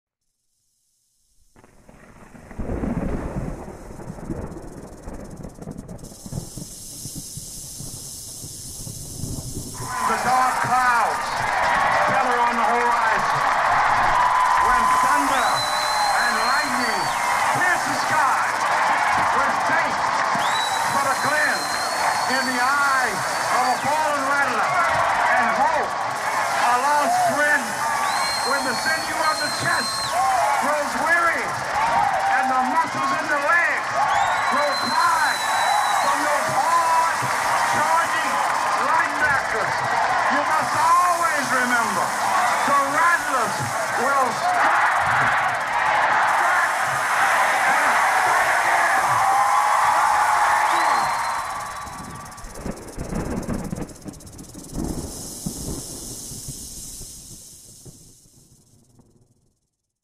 FAMU Rattler Charge
Any hardcore Rattler knows the Charge! Dr. Frederick S. Humphries recites the fiery proclamation in front of the faithful Rattler Nation at Bragg Stadium. I added the words to go along with it. Some theatrics are added of course for some flavor!